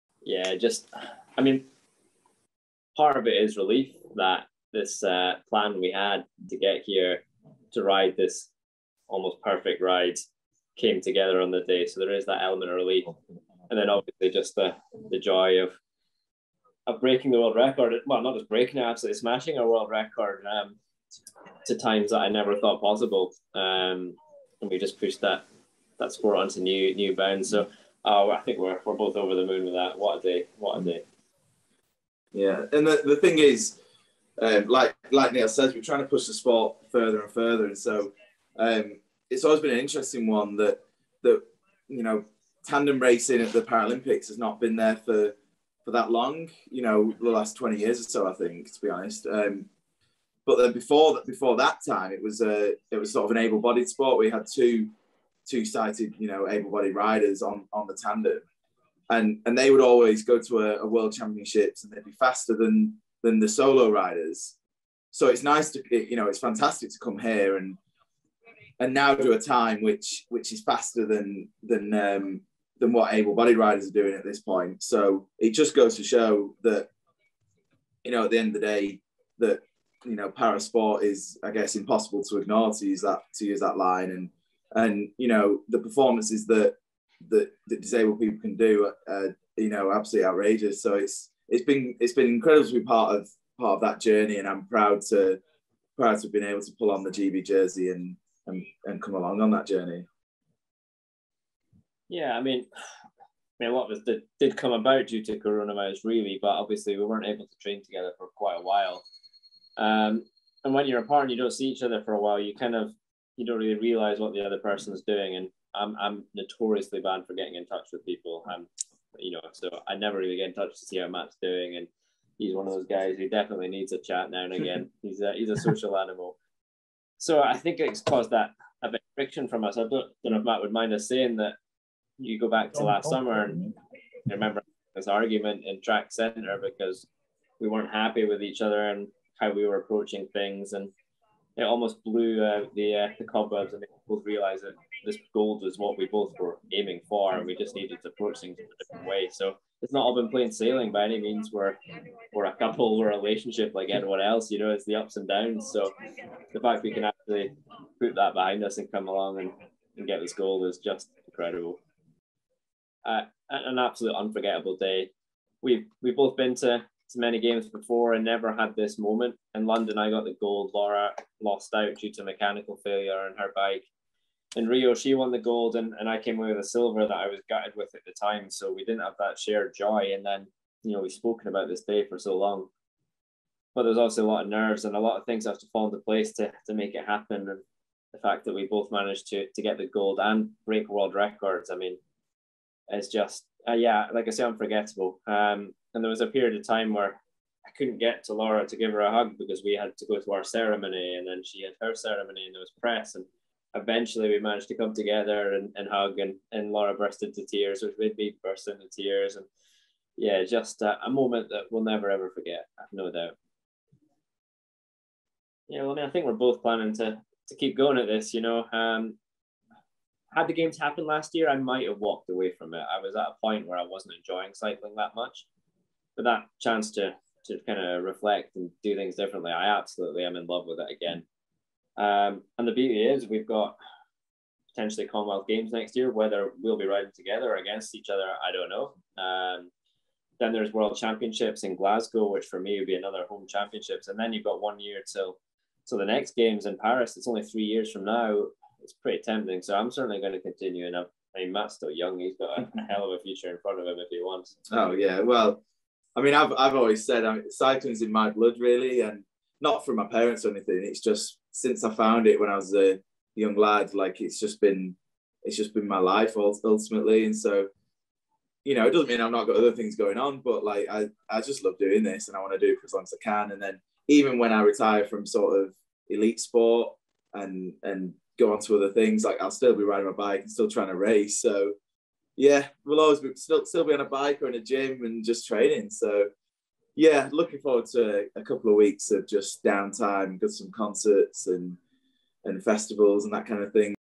'Perfect ride' lead to Neil Fachie and Matthew Rotherham's gold in men's B1000m time trial
Neil Fachie and Matthew Rotherham on their gold in the B 1000m time trial, where they also smashed their own record.

The pair describe the challenges they have overcome during their training camp, where at one point they nearly fell out completely in training.

Neil also shares the experience of his wife Lora winning gold at the same games.